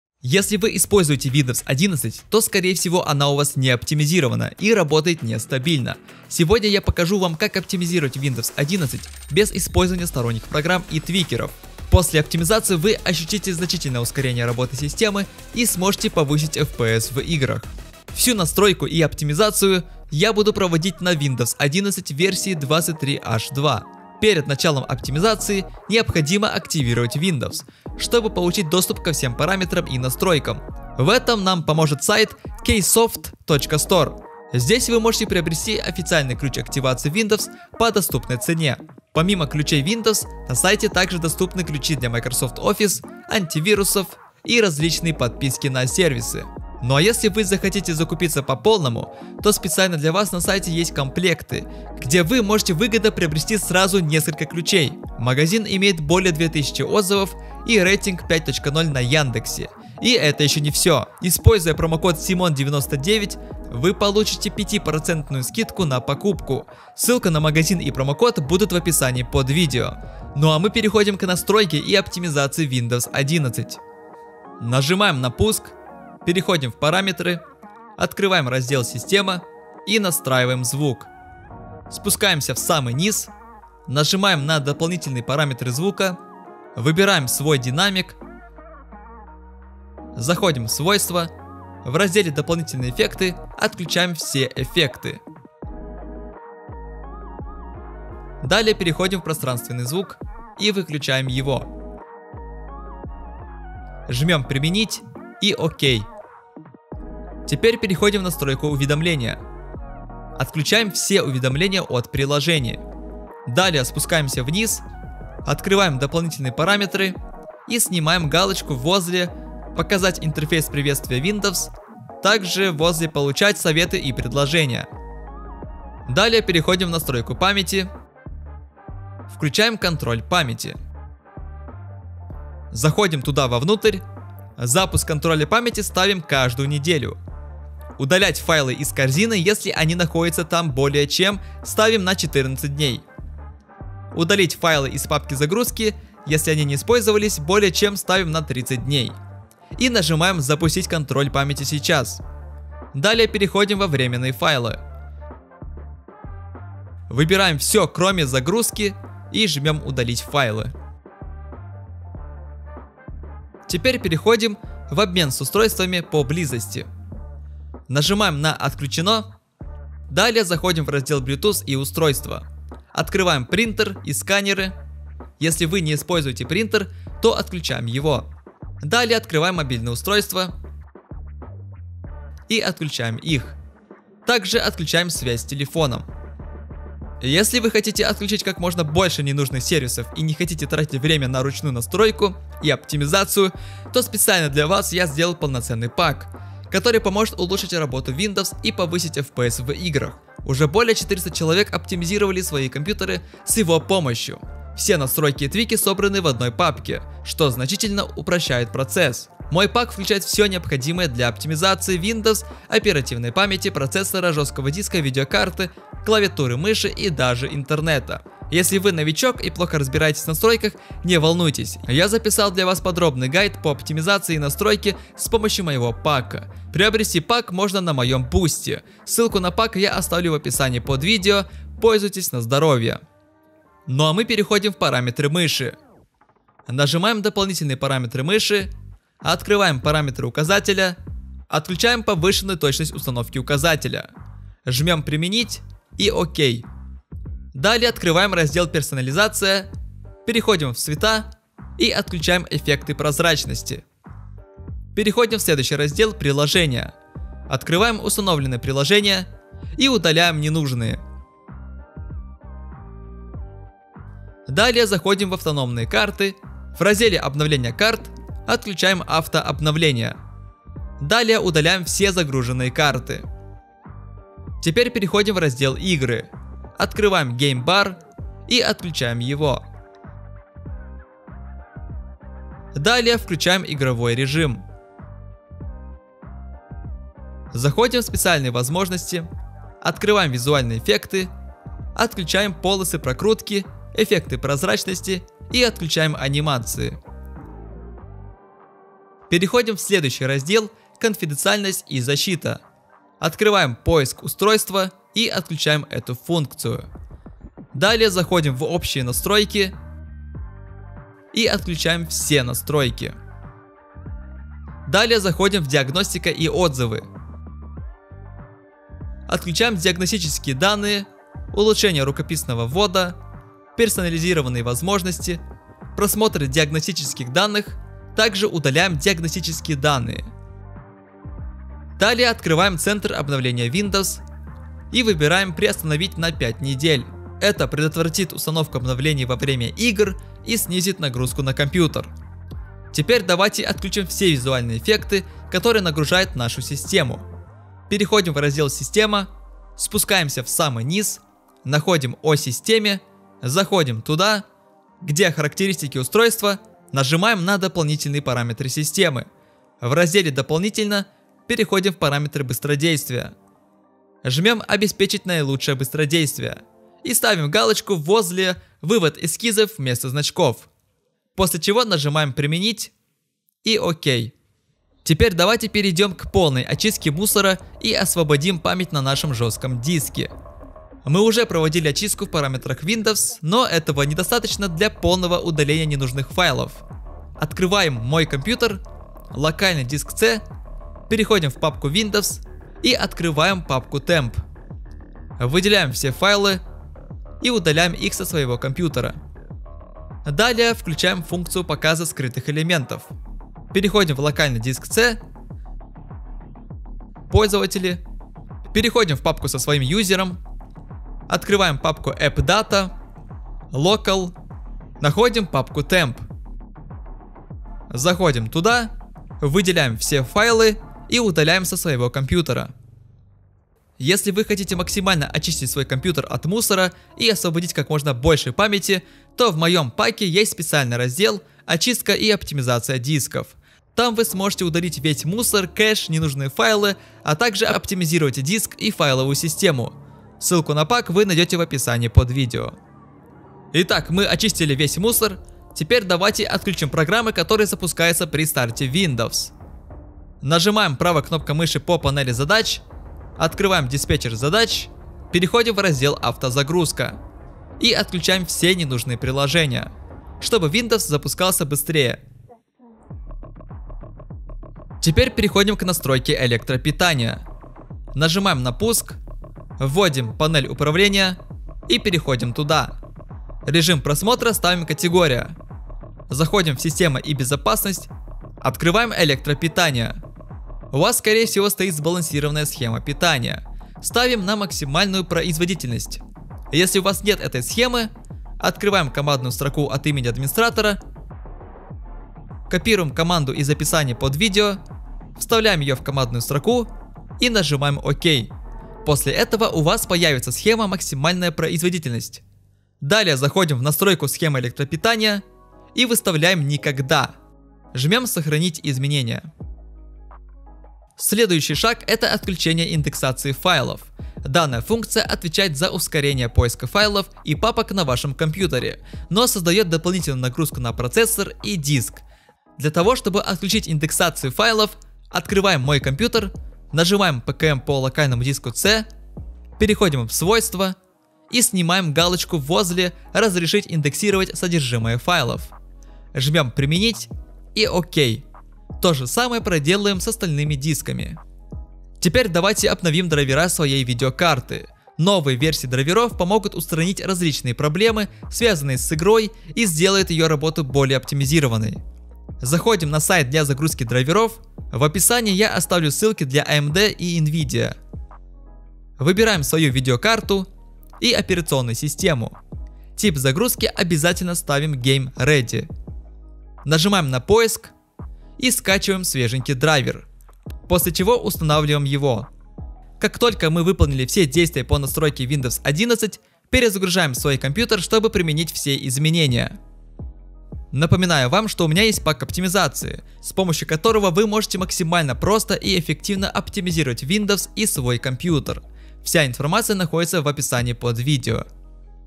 Как Поднять ФПС и Оптимизировать Windows 11 БЕЗ ПРОГРАММ и ТВИКЕРОВ? Оптимизация Винды Руками 2025
Покупай лецинзионные ключи 👇🏻
📈 Мой пак оптимизации для Буста ФПС 👇🏻
Если вы используете Windows 11, то, скорее всего, она у вас не оптимизирована и работает нестабильно. Сегодня я покажу, как оптимизировать Windows 11 без использования сторонних программ и твикеров. После оптимизации вы ощутите значительное ускорение работы системы и сможете повысить FPS в играх. Полная оптимизация windows 11. Гайд на настройку и оптимизацию windows 11. оптимизация виндовс 2025